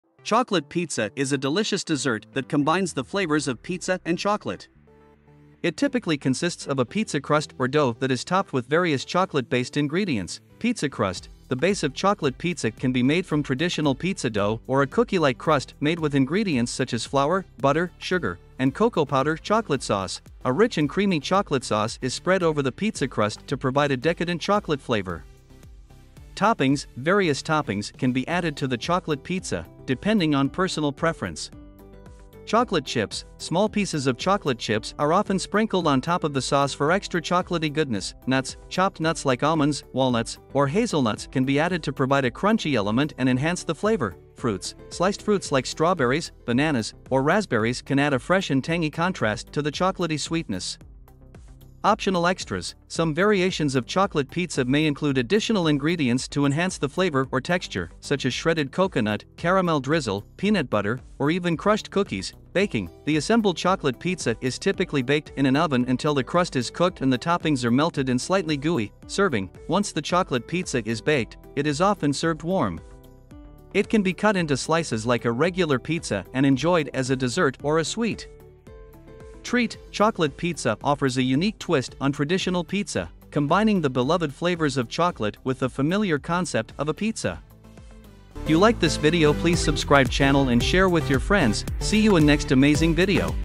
Chocolate pizza reviews | Best pizza must taste
Chocolate pizza is a delicious dessert that combines the flavors of pizza and chocolate. It typically consists of a pizza crust or dough that is topped with various chocolate-based ingredients. While there are different variations of chocolate pizza, here are some common details and ingredients:

1. Pizza Crust: The base of chocolate pizza can be made from traditional pizza dough or a cookie-like crust made with ingredients such as flour, butter, sugar, and cocoa powder.

2. Chocolate Sauce: A rich and creamy chocolate sauce is spread over the pizza crust to provide a decadent chocolate flavor. This sauce is typically made by melting chocolate and mixing it with cream or condensed milk.

3. Toppings: Various toppings can be added to the chocolate pizza, depending on personal preference. Here are some popular options:

   - Chocolate Chips: Small pieces of chocolate chips are often sprinkled on top of the sauce for extra chocolatey goodness.

   - Marshmallows: Miniature marshmallows are a common topping that adds a fluffy and sweet texture to the pizza.

   - Nuts: Chopped nuts like almonds, walnuts, or hazelnuts can be added to provide a crunchy element and enhance the flavor.

   - Fruits: Sliced fruits like strawberries, bananas, or raspberries can add a fresh and tangy contrast to the chocolatey sweetness.

4. Optional Extras: Some variations of chocolate pizza may include additional ingredients to enhance the flavor or texture, such as shredded coconut, caramel drizzle, peanut butter, or even crushed cookies.

5. Baking: The assembled chocolate pizza is typically baked in an oven until the crust is cooked and the toppings are melted and slightly gooey. Baking time and temperature can vary depending on the recipe.

6. Serving: Once the chocolate pizza is baked, it is often served warm. It can be cut into slices like a regular pizza and enjoyed as a dessert or a sweet treat.

Chocolate pizza offers a unique twist on traditional pizza, combining the beloved flavors of chocolate with the familiar concept of a pizza. It is a popular dessert option at parties, gatherings, and pizzerias that cater to sweet-toothed individuals.